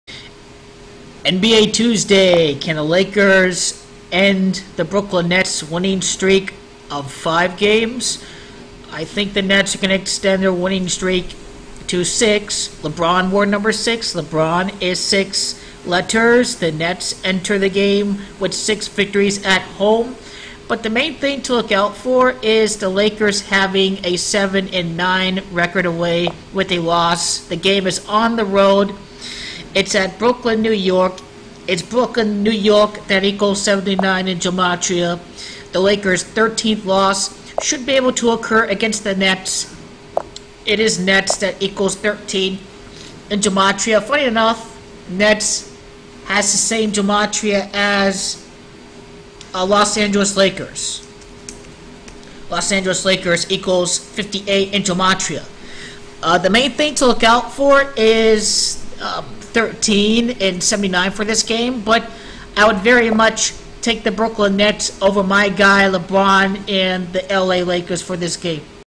NBA Tuesday, 12/18/18: Brooklyn Nets will beat LeBron James-LA Lakers to Extend Win Streak to 6!
Los Angeles will lose game #13 to the Brooklyn Nets away from home, and it is the Lakers that will ultimately pick up the 3rd seed in the west.